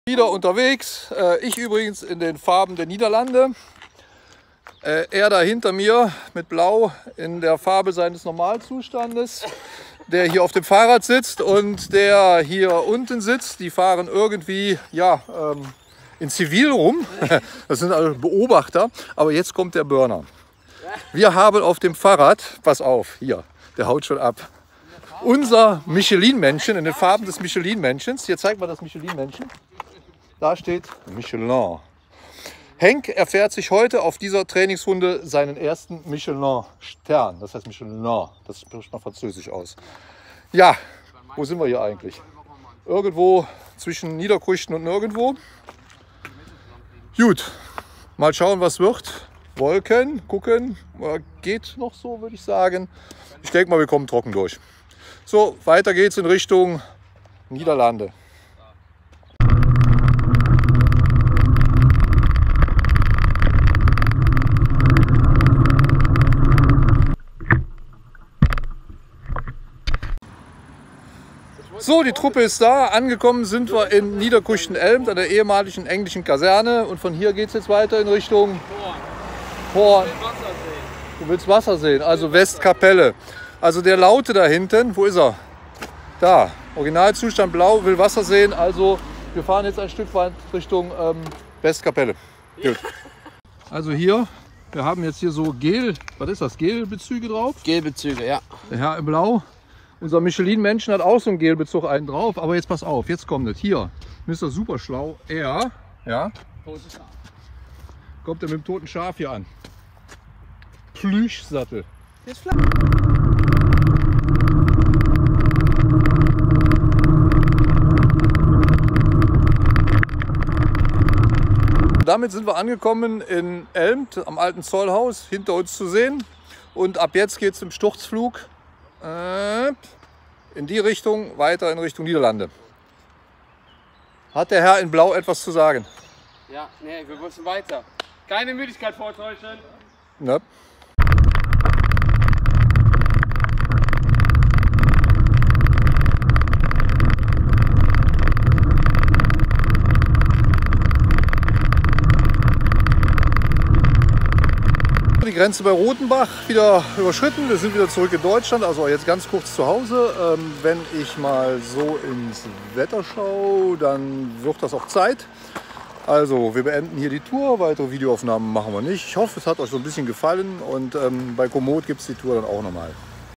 Radtour: Rund um den Nationaalpark "De Meinweg" (Komoot gelistet)
Hier findet ihr zunächst einmal unsere heutige - 50 Kilometer lange Trainingsroute - auf Komoot -
(die man auch mit einem normalen Fahrrad bewältigen kann)

In den kommenden Tagen werden wir vor allem die Wetterbedingungen im Auge halten. Ein klein wenig richtet sich unser Start auch nach dem Wetter.

Ein Dankeschön an dieser Stelle an den Drahtesel Ratheim ... und an Davids Dorflädchen ... Support ist auch in Trainingszeiten alles :-)

So - und hier unser Video vom Tag. Wie ihr seht hat es neben Training auch viel Spass gemacht Fahrradfahren macht anscheinend nicht nur die Muskeln locker - sondern strapaziert auch die Lachmuskulatur .